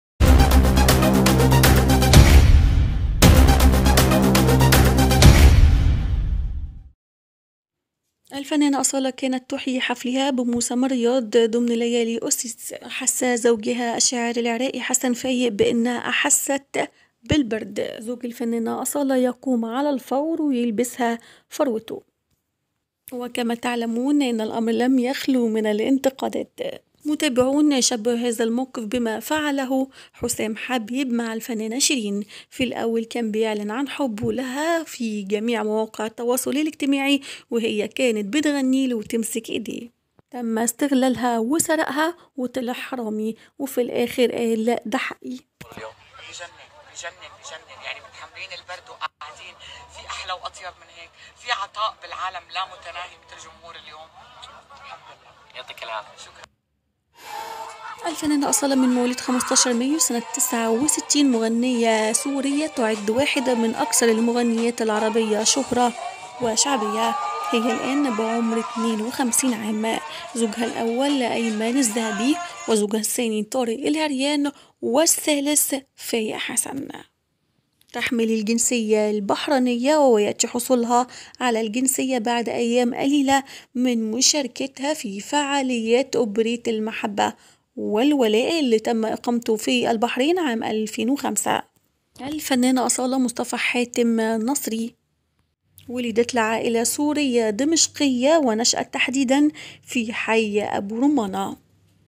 زوج أصالة نصري يلبسها فروته أثناء حفل اويسسس خوفا عليها من البرد وامام الجمهور
زوج الفنانة أصالة نصري حسن فائق يلبسها فروته أمام الجمهور وأثناء الحفلة خوفا عليها من البرد